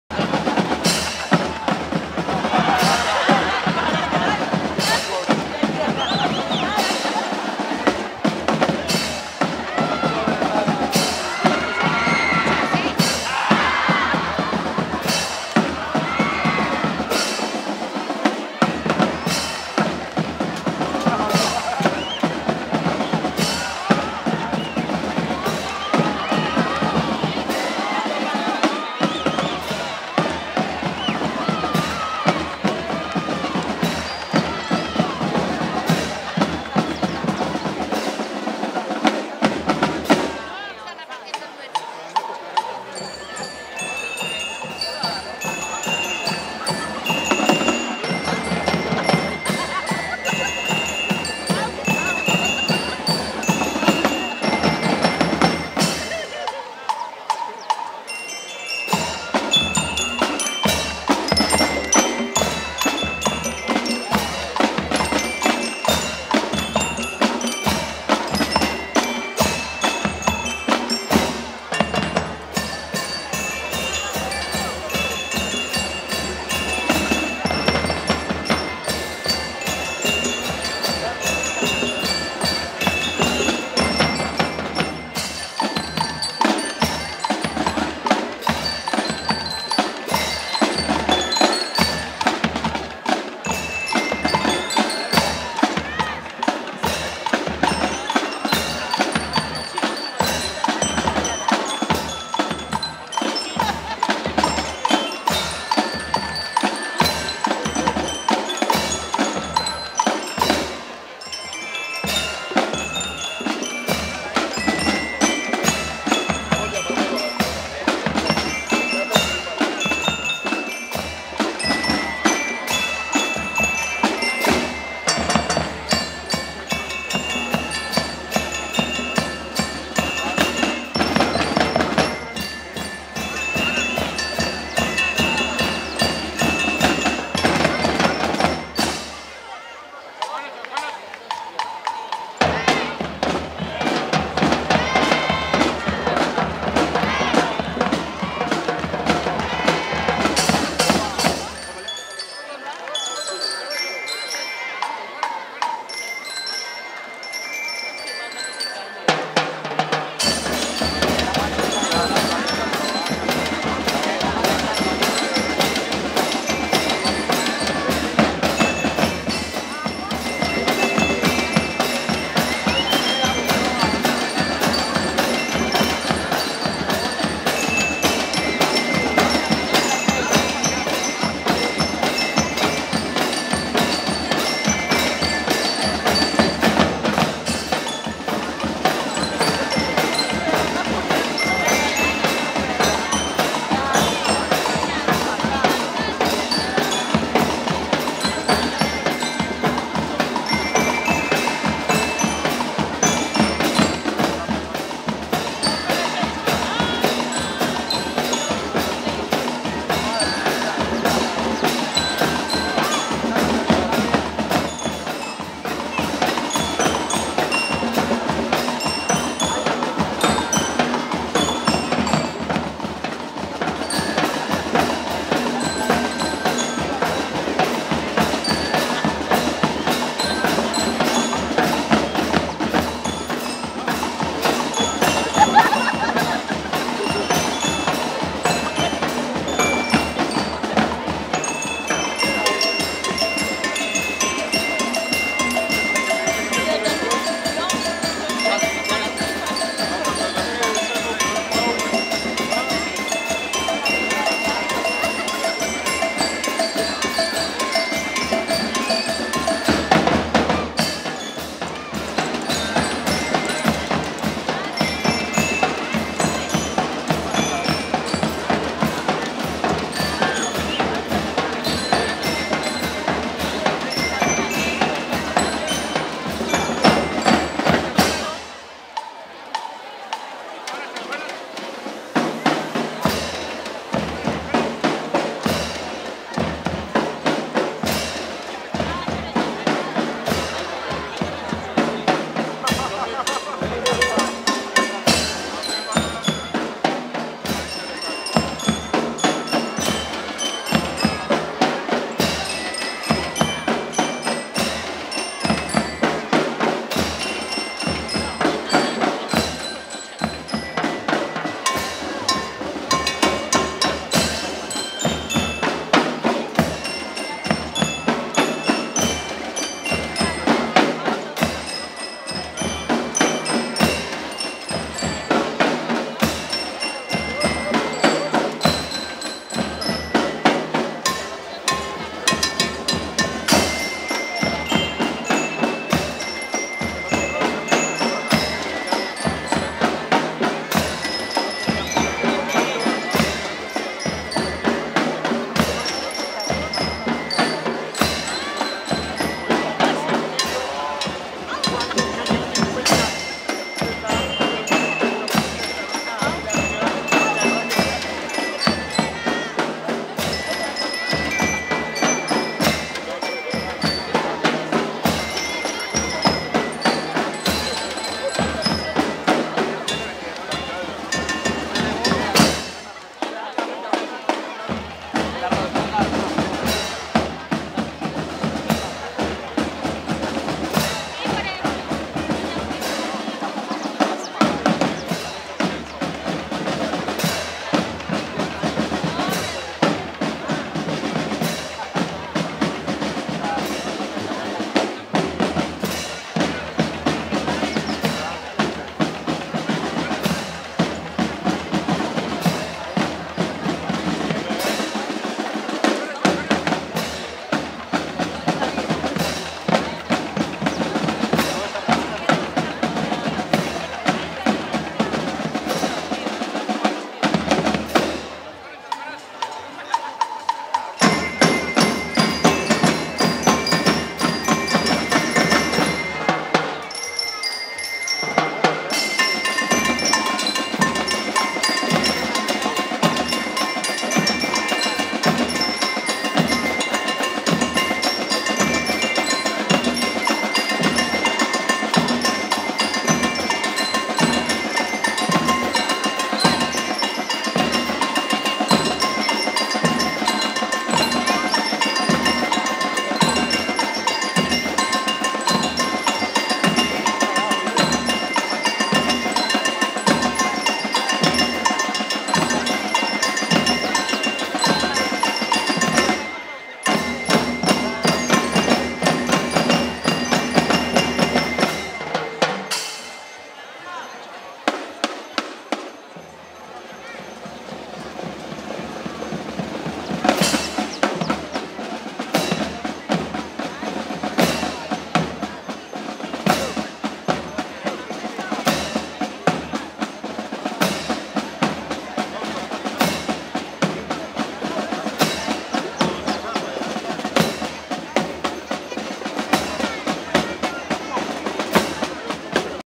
Banda Gimnasio Moderno de Buga | Calarca 2019
8vo Concurso Nacional de Bandas de Marcha Paisaje Cultural Cafetero, Calarcá Quindío 2019